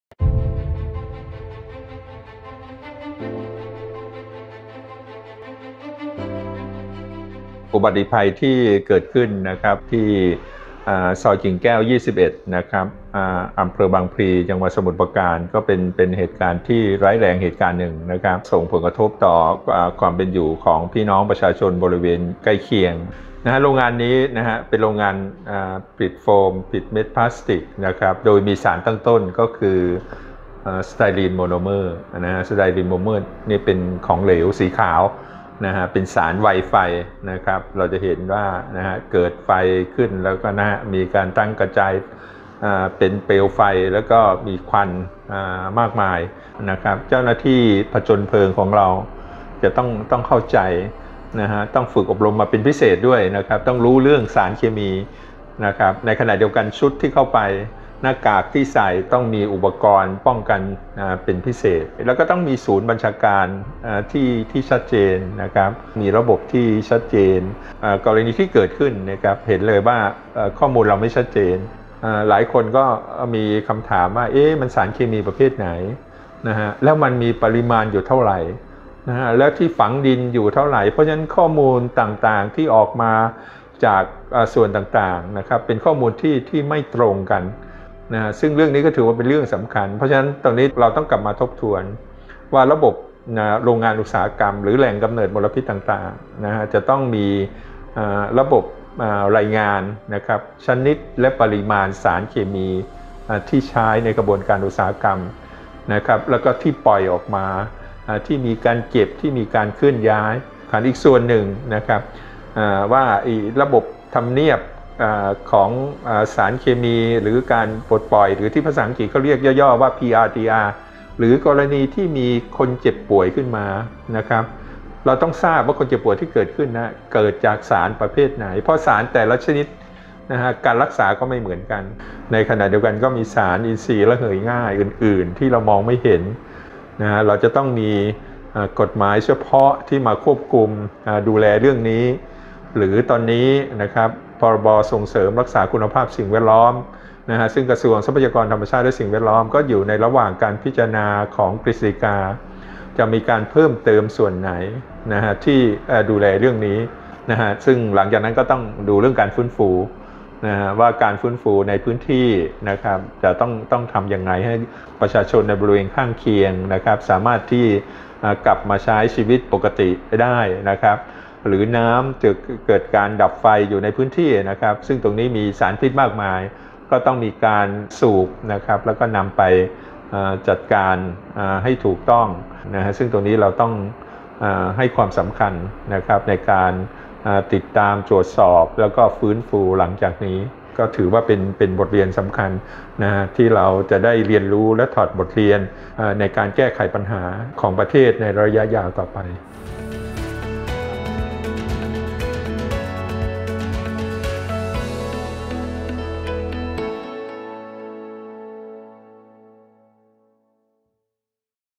มาตรการสิ่งแวดล้อมและความปลอดภัยที่ต้องทบทวนและจริงจัง
มาตรการสิ่งแวดล้อมและความปลอดภัยที่ต้องทบทวนและจริงจัง บทเรียนจากกรณีเพลิงไหม้โรงงานสารเคมี ย่านกิ่งแก้ว จ.สมุทรปราการ

ดร.วิจารย์ สิมาฉายา ผู้อำนวยการสถาบันสิ่งแวดล้อมไทย

👍 ติดตามข่าวสารดีๆ จาก TEI 🌳